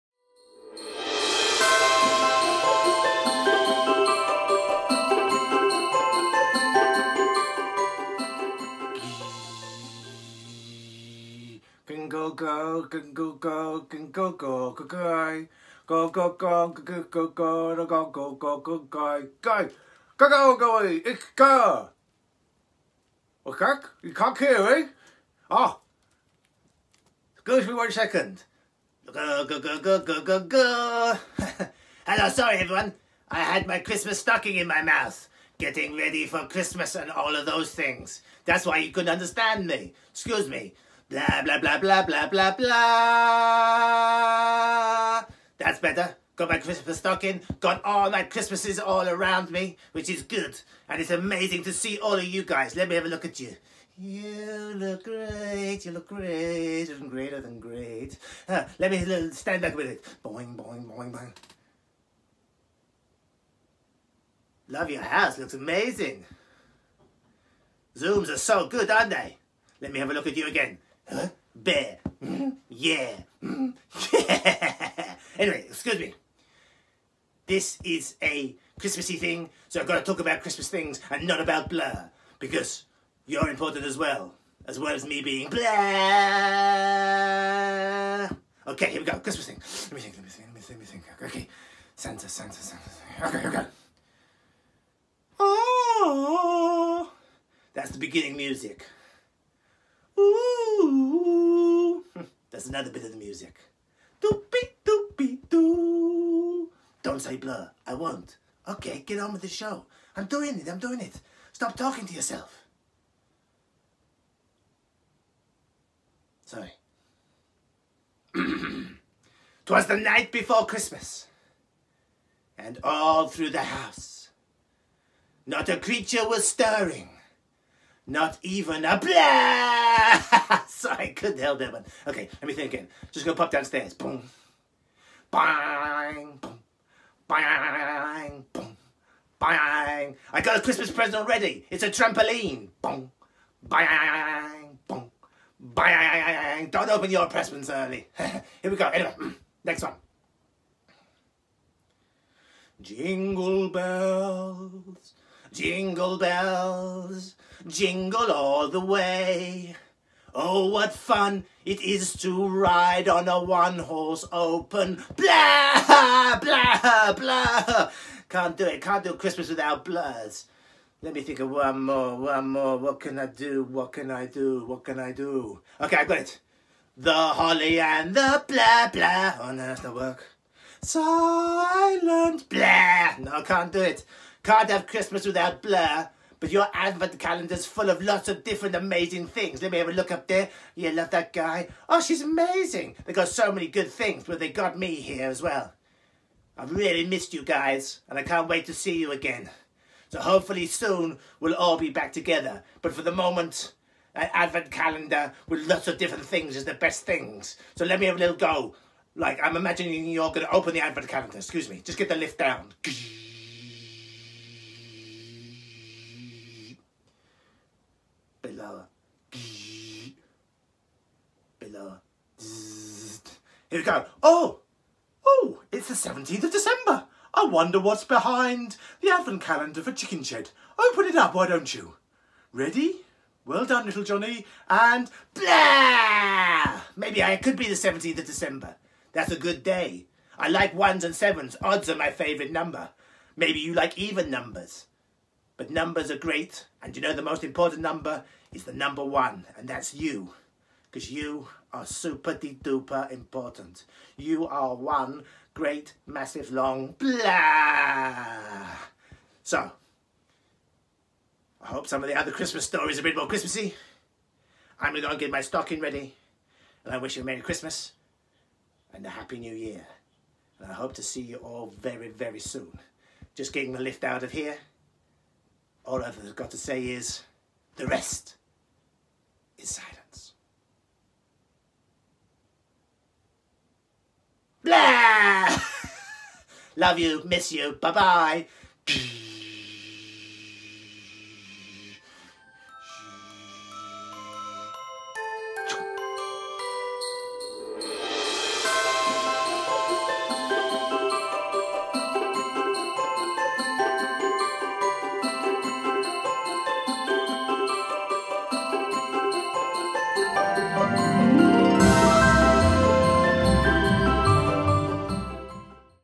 Blurgh's Christmas Message | 5th December
Chickenshed's very own Blurgh has special Christmas Message for you!